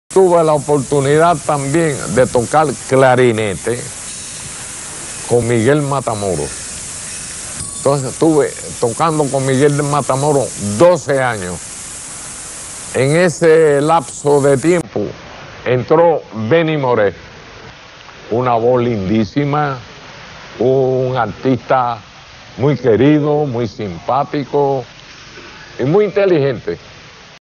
Benny More Habla Compay Segundo
compay segundo tocaba clarinete con el trio matamoros ahi conocio a benny more